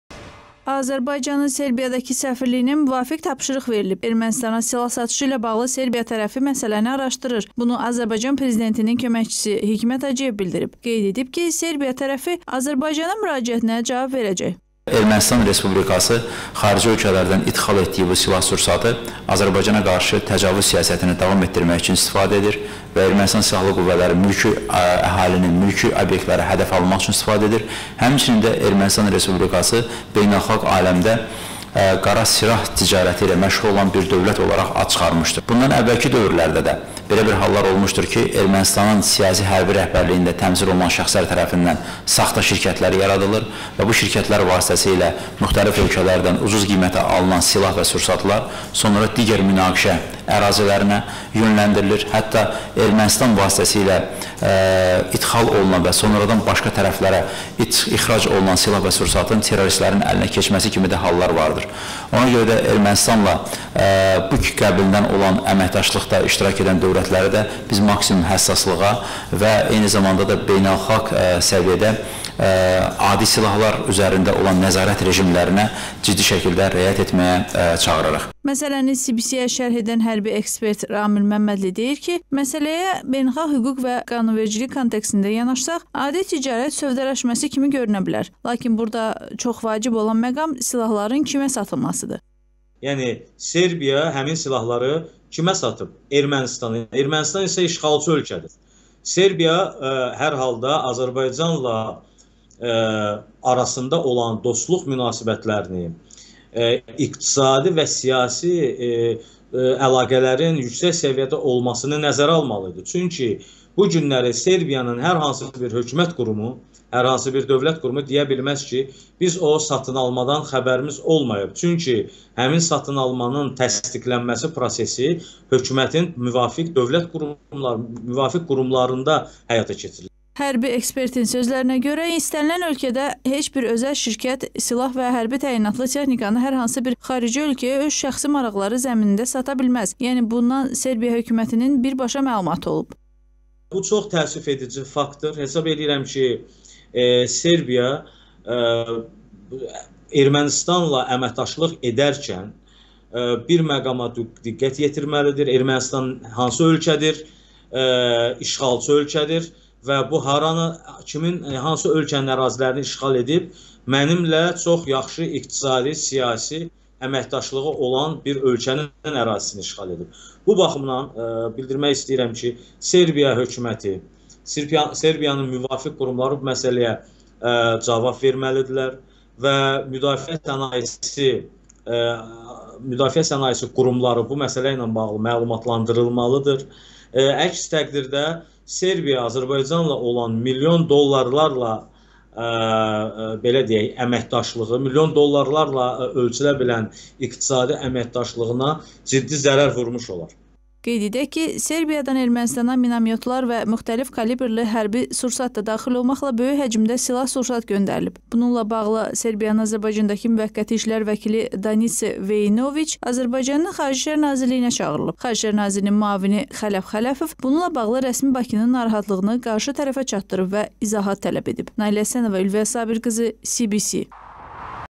HiKMƏT HACIYEV: “SERBiYA TƏRƏFi MƏSƏLƏNi ARAŞDIRIR”
“Azərbaycanın Serbiyadakı səfirliyinə müvafiq tapşırıq verilib. Ermənistana silah satışı ilə bağlı Serbiya tərəfi məsələni araşdırır”. Bunu Azərbaycan Prezidentinin köməkçisi Hikmət Hacıyev bildirib. Qeyd edib ki, Serbiya tərəfi Azərbaycanın müraciətinə cavab verəcək.
Məsələni CBC-yə şərh edən hərbi ekspert Ramil Məmmədli deyir ki, məsələyə beynəlxalq hüquq və qanunvericilik kontekstində yanaşsaq, adi ticarət sövdələşməsi kimi görünə bilər. Lakin burada çox vacib olan məqam silahların kimə satılmasıdır.
Hərbi ekspertin sözlərinə görə, istənilən ölkədə heç bir özəl şirkət silah və hərbi təyinatlı texnikanı hər hansı bir xarici ölkəyə öz şəxsi maraqları zəminində sata bilməz. Yəni, bundan Serbiya hökumətinin birbaşa məlumatı olub.
Qeyd edək ki, Serbiyadan Ermənistana minomyotlar və müxtəlif kalibrli hərbi sursat da daxil olmaqla, böyük həcmdə silah-sursat göndərilib. Bununla bağlı Serbiyanın Azərbaycandakı müvəqqəti işlər vəkili Danitsa Veinoviç Azərbaycanın Xarici İşlər Nazirliyinə çağırılıb. Xarici işlər nazirinin müavini Xələf Xələfov bununla bağlı rəsmi Bakının narahatlığını qarşı tərəfə çatdırıb və izahat tələb edib.